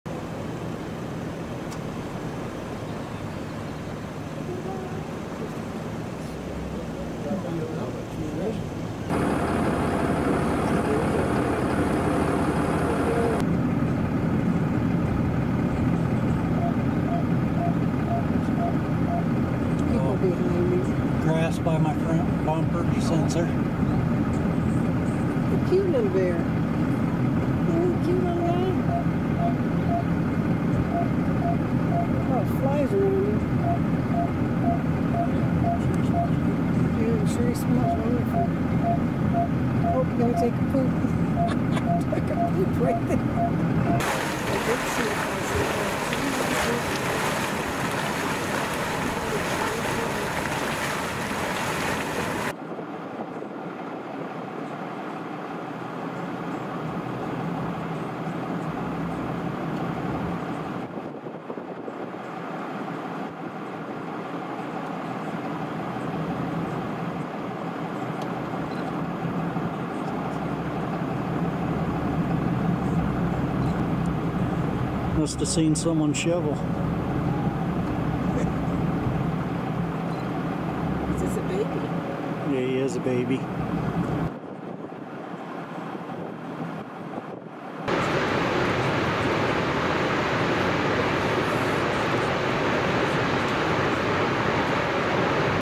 Yellowstone National Park - Bear and Wolf
This video shows a bear and wolf in Yellowstone National Park. It also has a couple of shots of Yellowstone scenery.